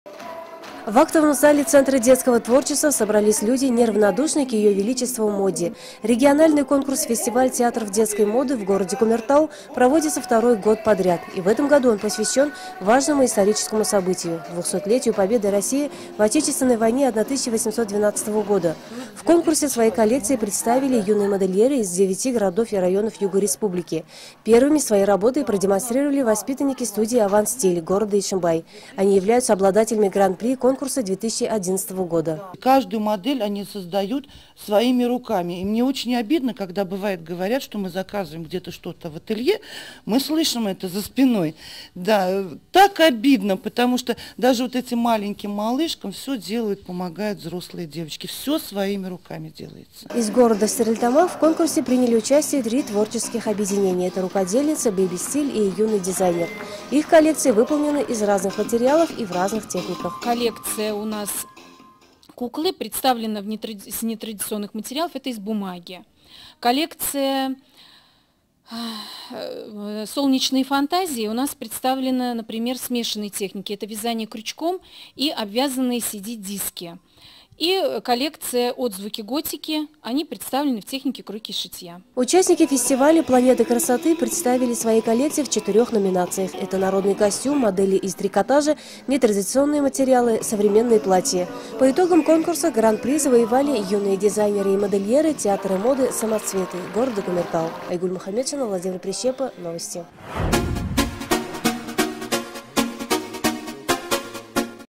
Планета красоты.mp4
В городе Кумертау прошел второй региональный фестиваль Театров детско-юношеской моды «Планета красоты». Свои коллекции представили как участники первого фестиваля, так и дебютанты. Фестиваль проводится в Кумертау второй год подряд. В этом году он посвящен 200-летию Победы России в Отечественной войне 1812 года.